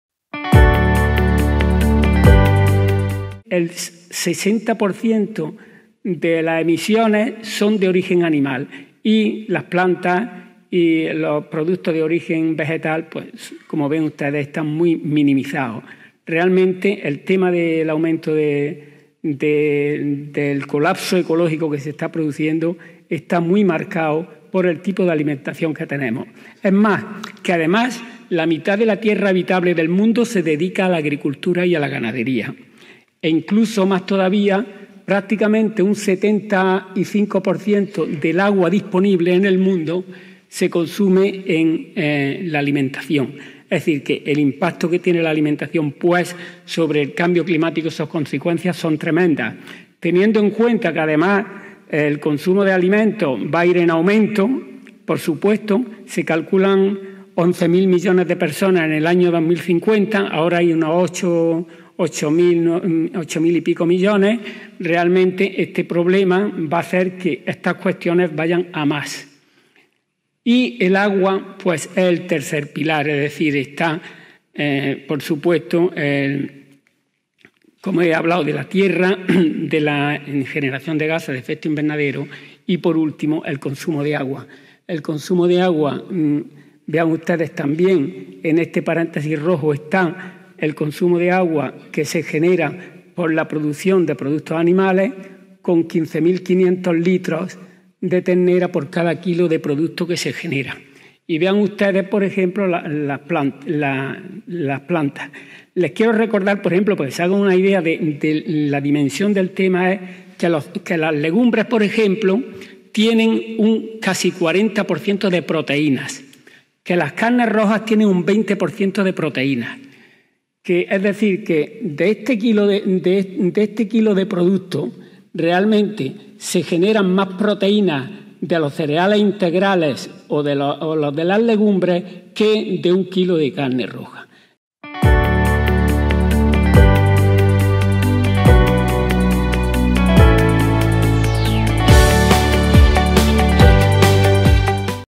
Francisco Pérez: "El médico tiene la responsabilidad de liderar la transición alimentaria"
El impacto del cambio climático en la salud humana es una de las principales preocupaciones de la Medicina Interna, tal y como se ha reflejado en la conferencia inaugural del 44º Congreso Nacional de Medicina Interna. Tal y como ha explicado Francisco Pérez Jiménez, catedrático emérito de la Universidad de Córdoba, “la consecuencia del cambio climático es la principal amenaza de salud de los humanos. La recomendación de una dieta planetaria debe limitar las proteínas animales y aumentar las vegetales”. Para conseguirlo, el catedrático ha afirmado ser partidario de “que los médicos tienen la responsabilidad de liderar esta transición alimentaria”.